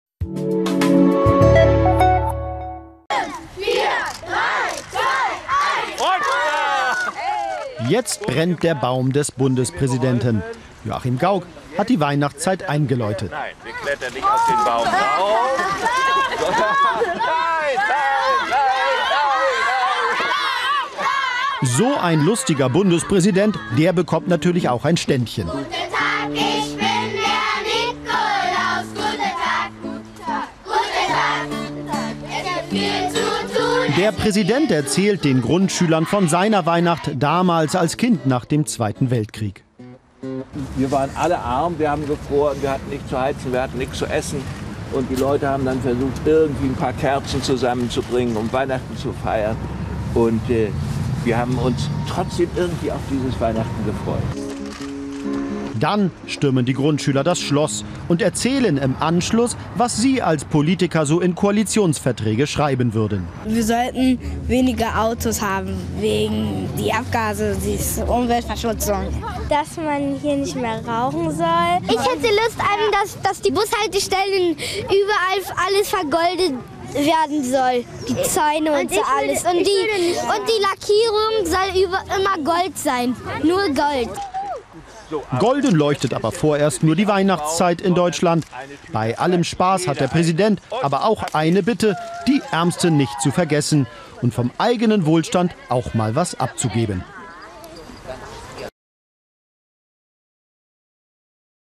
Bellevue-Baum brennt | Journal
Vor dem Berliner Schloss Bellevue wurde der Tannenbaum des Bundespräsidenten in Betrieb genommen. Da legt man nicht einfach den Lichtschalter um, das wird natürlich inszeniert - und dafür hat sich Joachim Gauck lautstarke Unterstützung geholt.